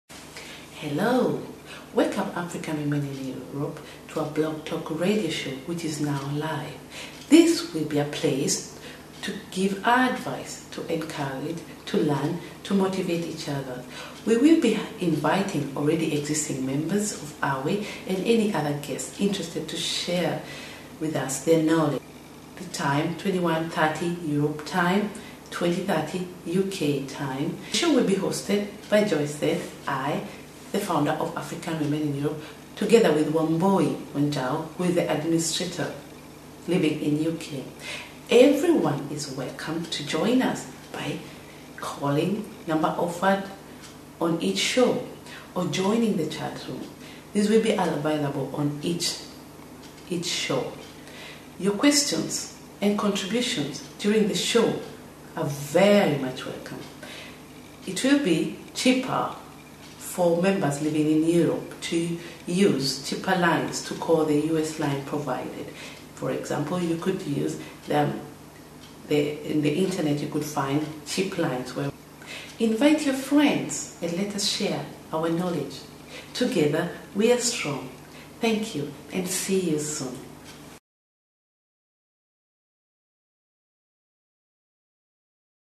African Women in Europe Blog Talk Radio
Blog talk radio created for African women in Europe . A private website
and for women around the world ,to motivate, to encourage and support
eachother